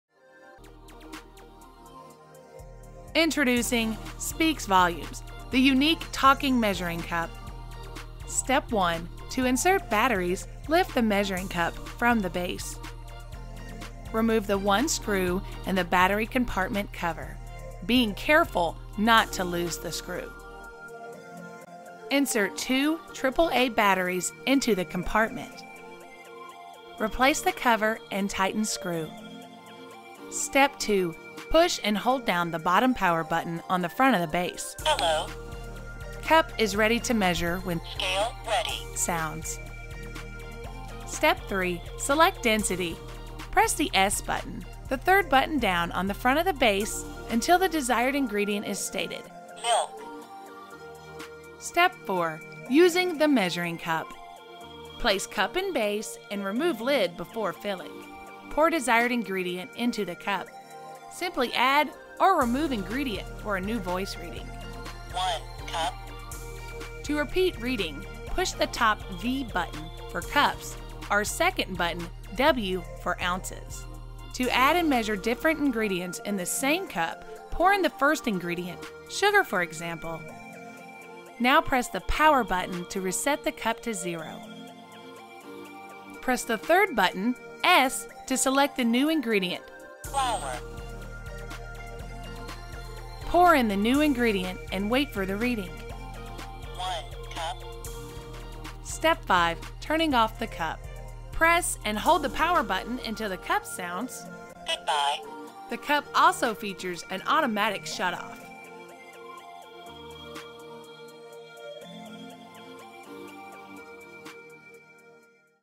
SpeaksVolumz Instructional Video
An easy to follow step by step guide to using the SpeaksVolumz Talking Cup by Meridian Home Products.